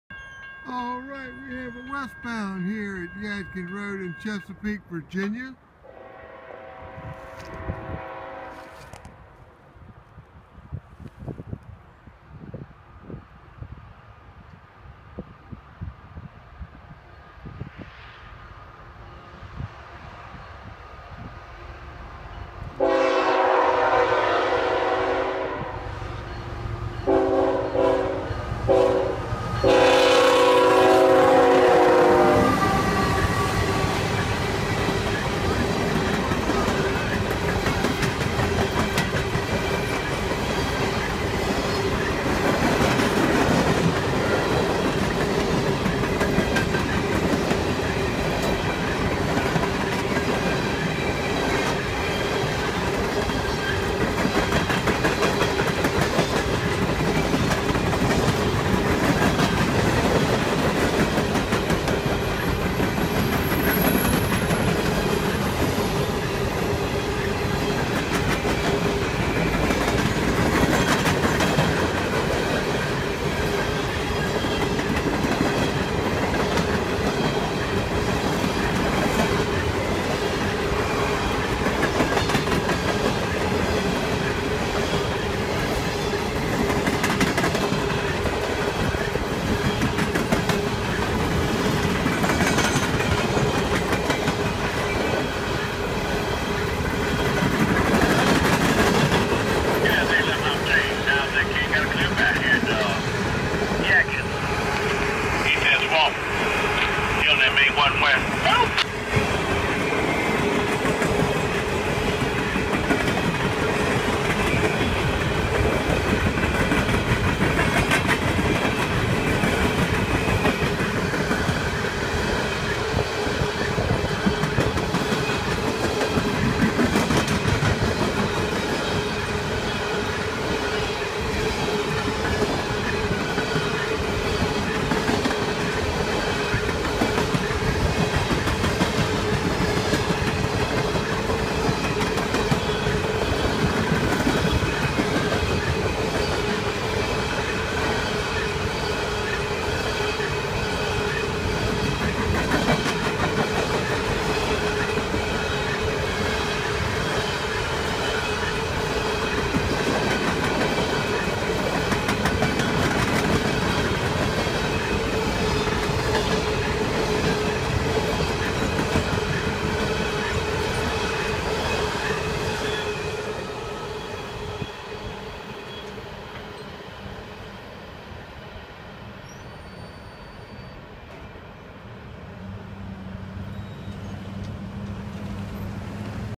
NS 3633 (GE ET44AC) leads NS 821
I love those Tier IV units. I didn't at first but now they have got into my blood. Here i catch NS 3633 (GE ET44AC) leading NS 821 west at Yadkin on 02/07/2017. Enjoy.